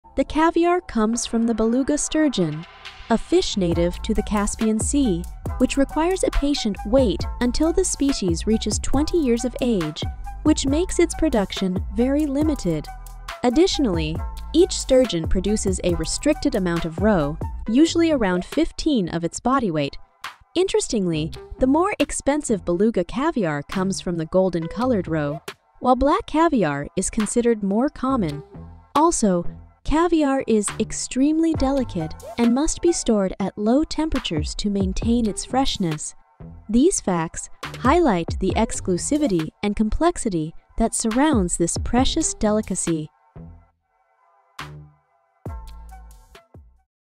Did you know this about caviar ?
Embark on a journey through the mysterious world of Beluga caviar, where limited production, golden roe, and delicate flavors captivate discerning palates.